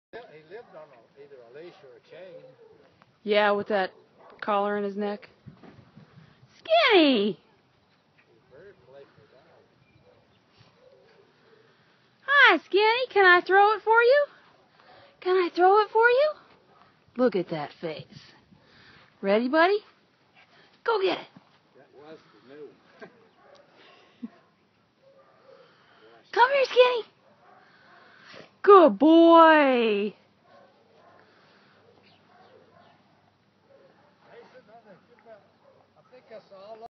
Skinny
Skinny, is a calm easy going Pit Bull Terrier. He is 5 years 9 months old. He has had a rough life, but has put it past him and he is ready to start the rest of his life with new people in it. He loves his tennis ball and plays with it quietly. He will sit for a treat and take it gently. He doesn't ask much of his people friends just a little of your time and love something he hasn't know much of. You can meet him at the Citrus County Animal Shelter. His adoption fee is $60 that includes him being fixed, up to date shots, Micro chipped, 30 day free pet insurance and Free obedience class. So come on over and meet Skinny.